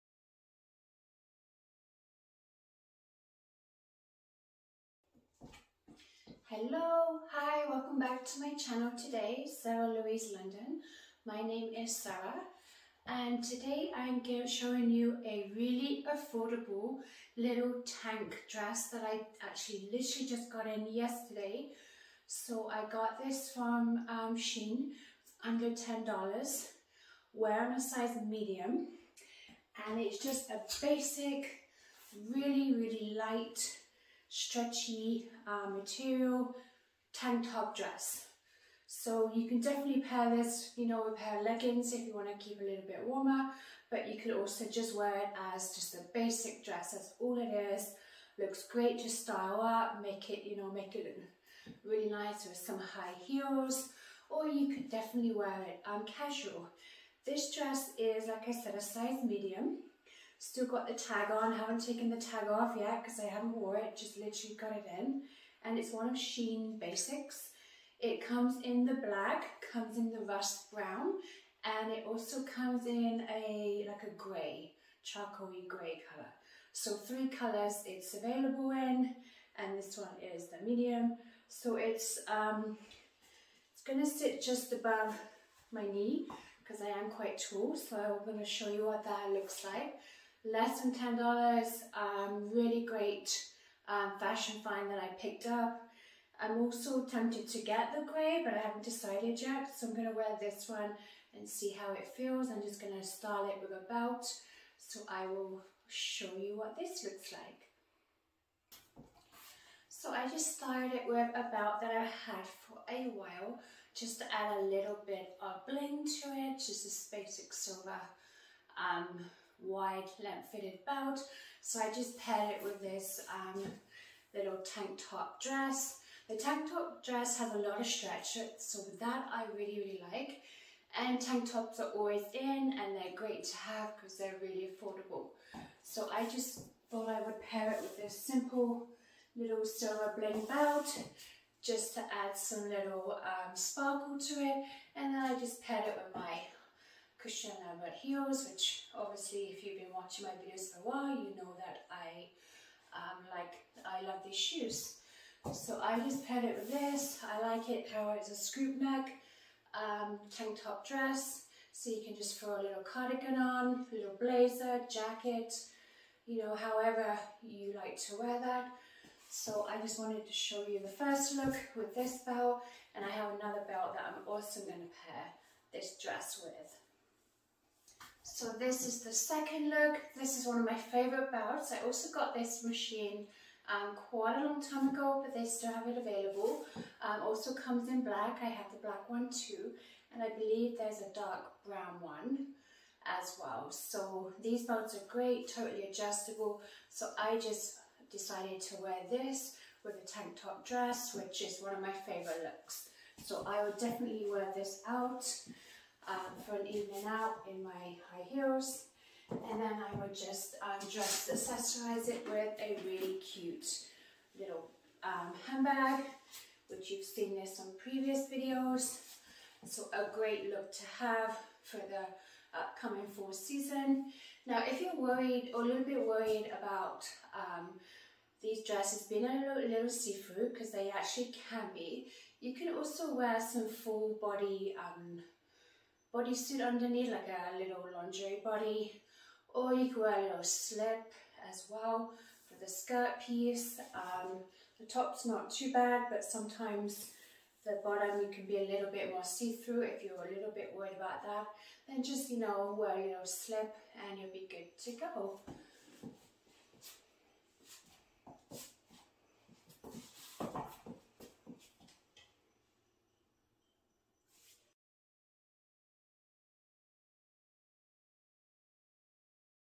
Little Black Dress, Tank top Dress
Sharing this little tank top jersey dress find today, affordable too and you can wear alone or with a pair of leggings, dress it up with your favourite belt or dress it down.
Just a simple dress available in 3 colors, lots of stretch to it too, soft material, wearing size medium.